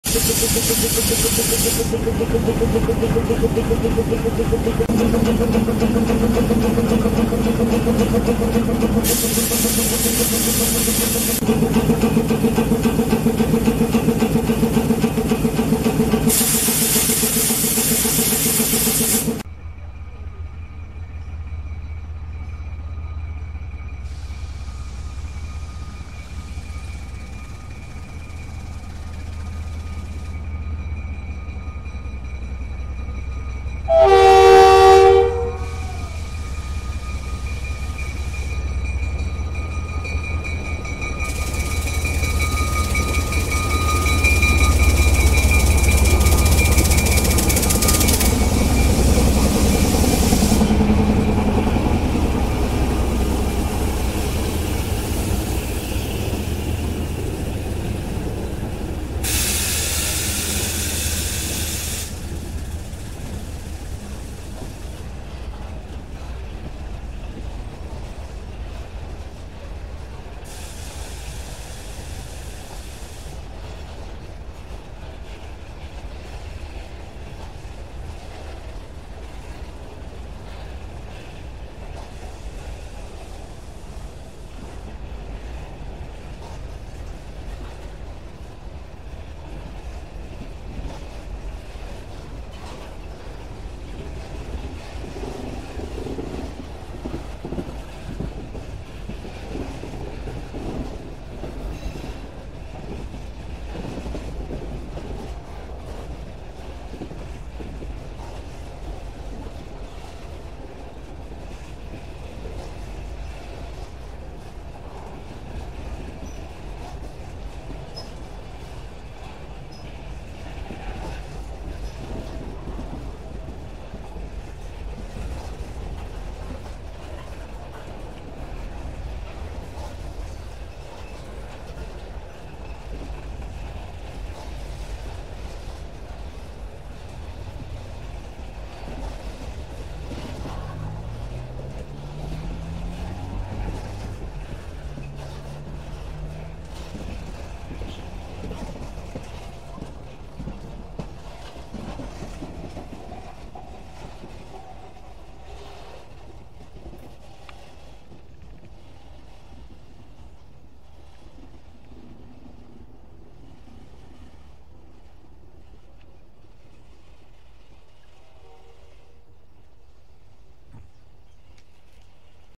LOCOMOTIVAS U20 ORQUESTRA DA MADRUGADA
3049 - Trio de Locomotivas U20 – Estação Dr. Lund – Pedro Leopoldo – MG, locomotivas 3811, 2505, 3814, em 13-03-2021, sábado, chegada aproximada 03:30 e partida às 07:58 hs, com 20 vagões tipo HFD carregados, trafegando sentido Belo Horizonte – MG.